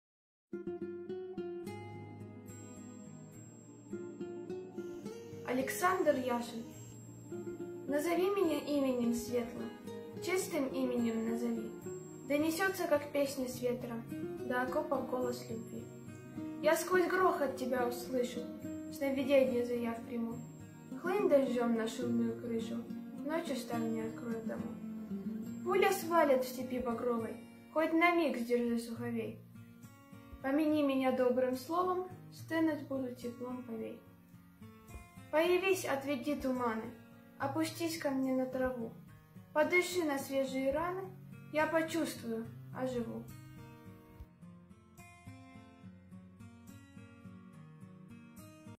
081 Конкурс! Стихотворение Александра Яшина "Назови меня именем светлым"
Читает Сара Аль Момани